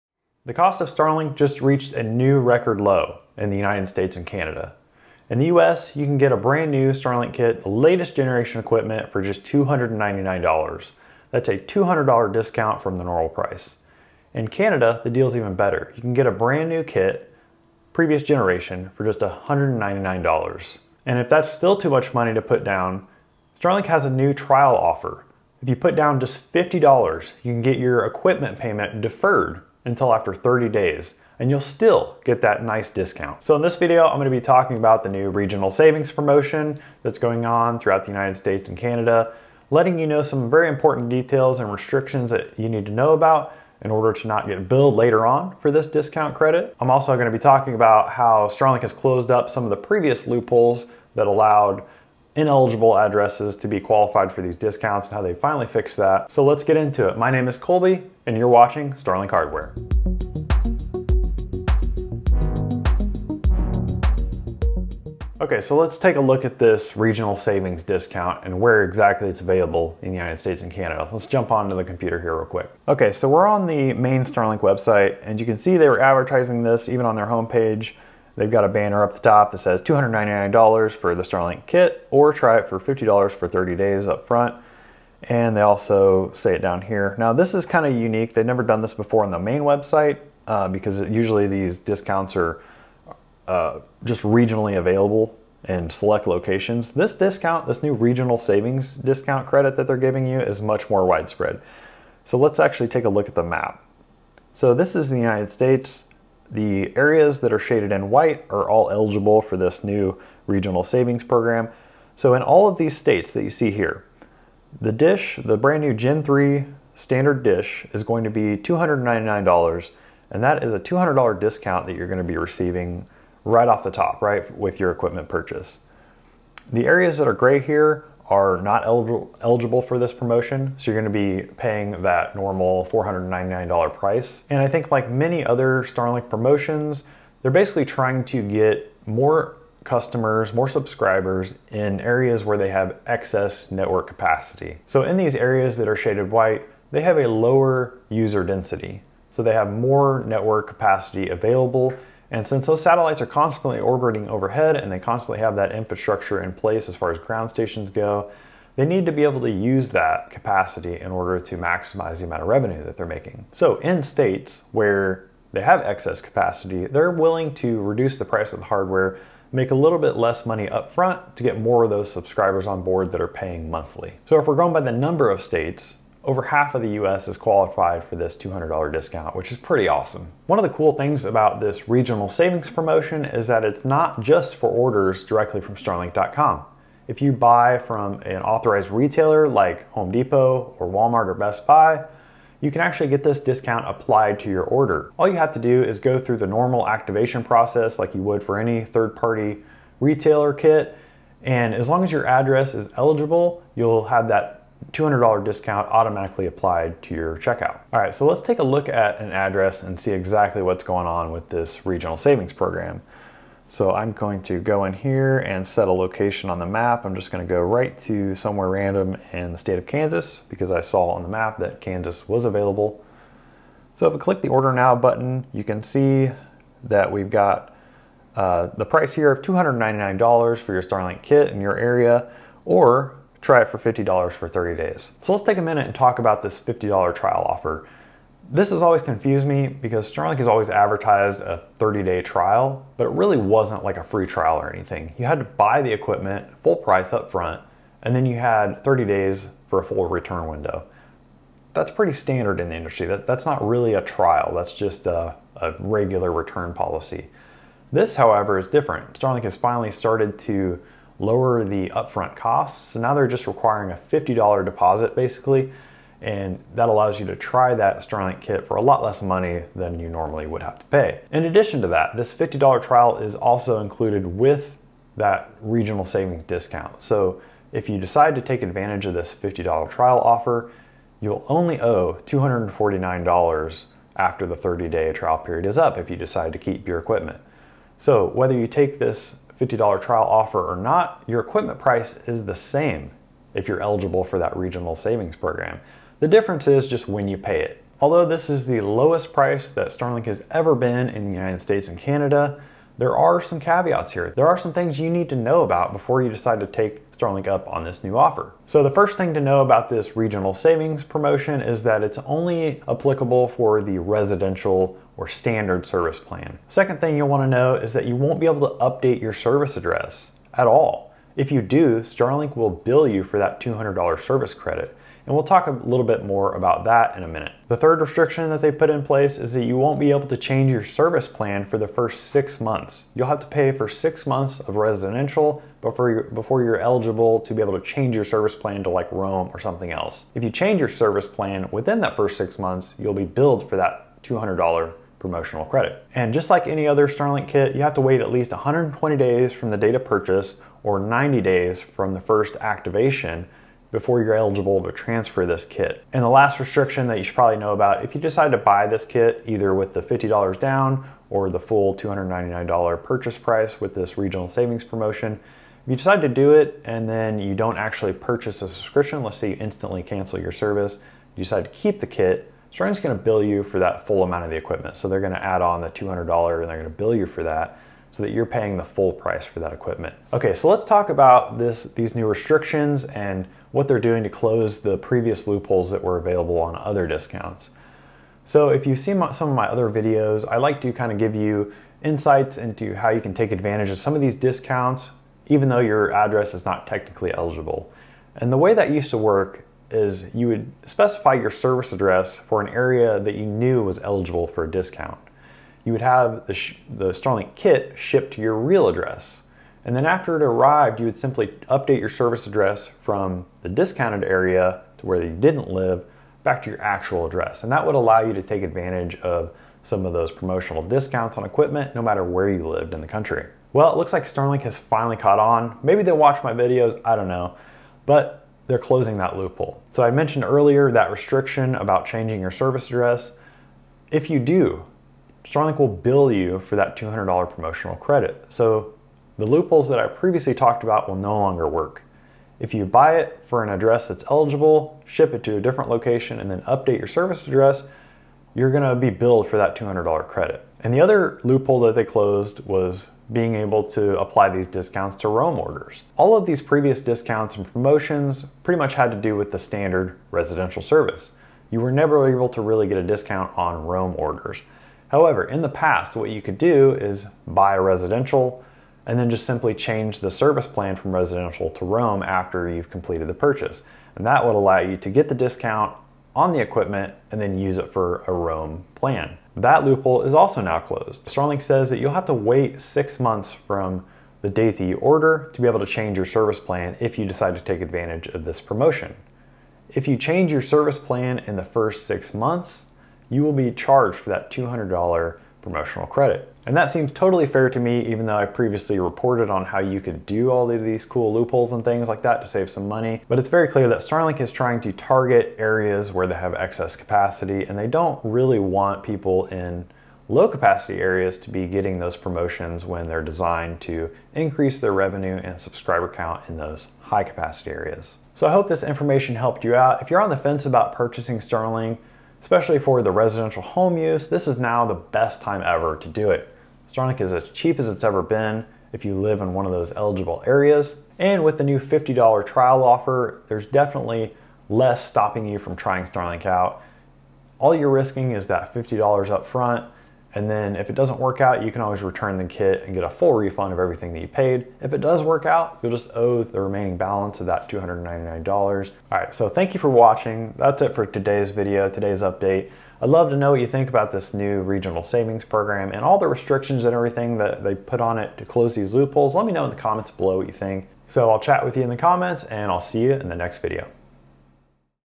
Starlink for $299?
Starlink reaches a record low price of just $299 in much of the United States and Canada. Still too much? New customers also have the option of a $50 trial. In this video, I explain all the details and important restrictions to know about before buying.

*My personal Starlink gear:*
eero Pro 6E Mesh System
TP-Link 8 port switch
Powerline adapters